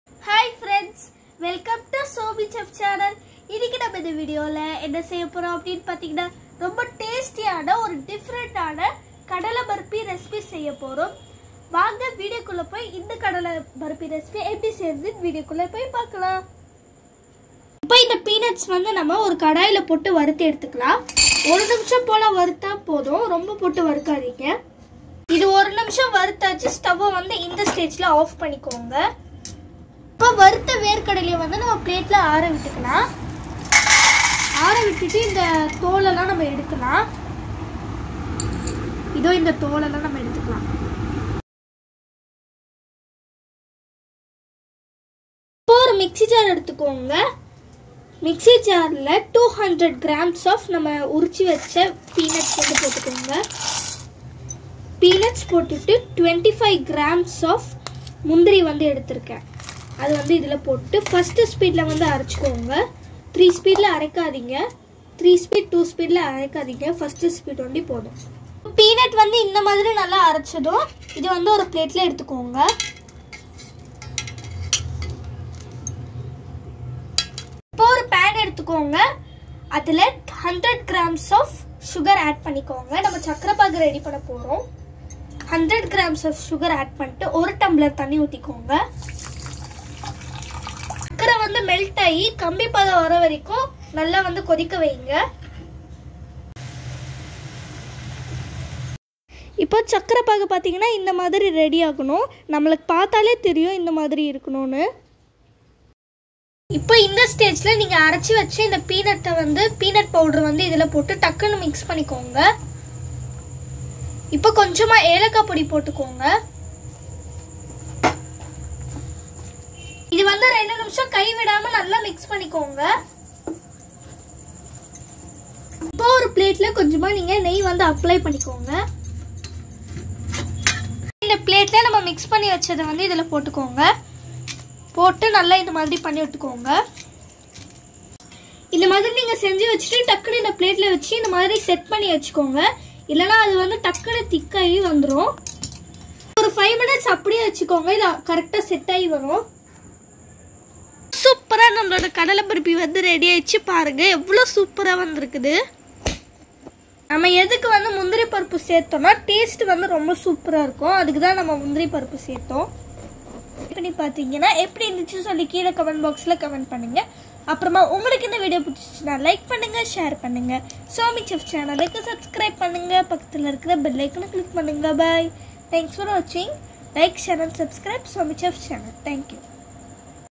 Peanut and cashewnut burfi/வேர்க்கடலை முந்திரி பர்ஃபி for Krishna jeyanthi.
Happy KRISHNA  JEYANTHI
peanut 200g
cashewnuts 25g
blend it
prepare sugar syrup
put 100g  sugar in a pan add 1 glass of water
boil it until kambi pada

add blended nuts
add cardamom powder  mix it for 2 minutes
pour it in ghee spreader plate keep it for 5 minutes

HAPPY Krishna jeyanthi.